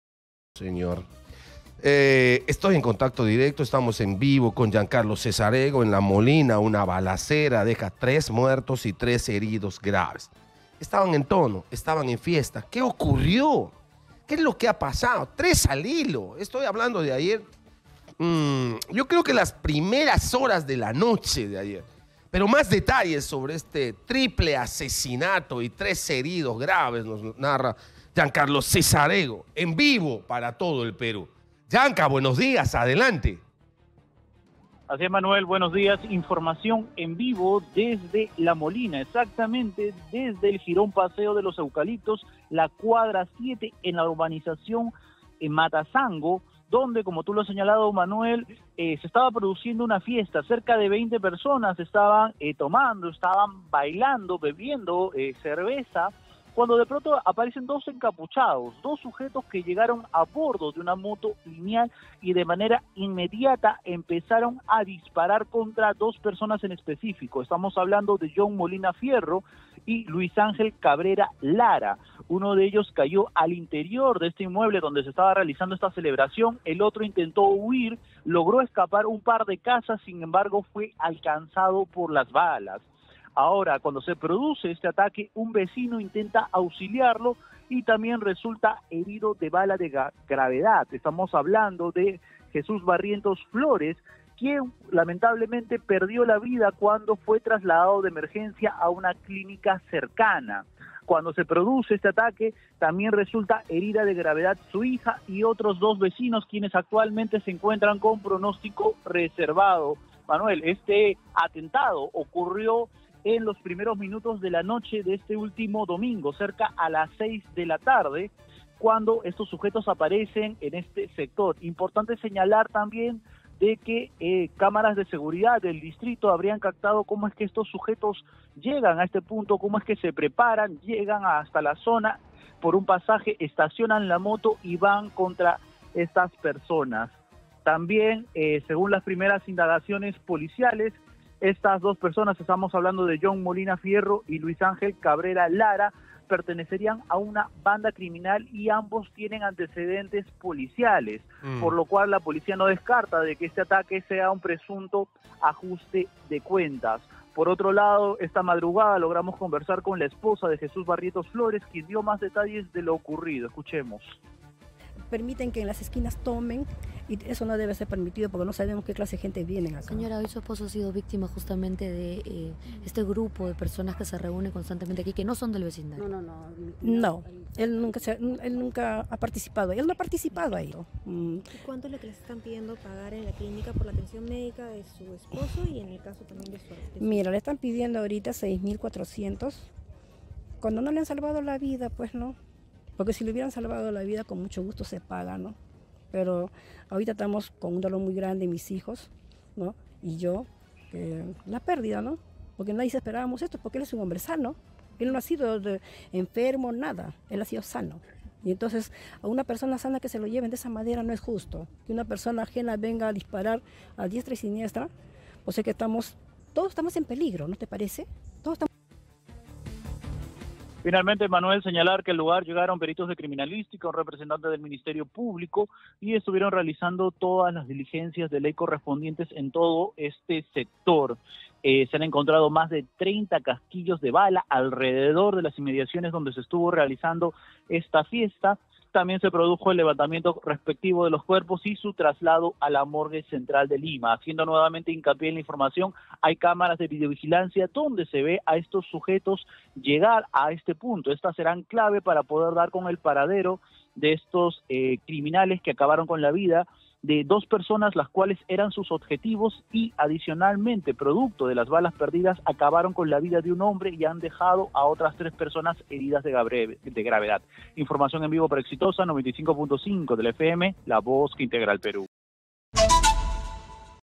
🔴🔵¡Trágico! Tres muertos y tres heridos deja feroz balacera en fiesta de La Molina
Más de 20 casquillos de bala fueron hallados en el asentamiento humano Matazango luego de que dos sicarios quitaran la vida a tres invitados de una fiesta en La Molina. Según se precisó, otras tres personas también resultaron heridas.

Exitosa Perú
"Exitosa: La voz que integra al Perú"

Corporación Universal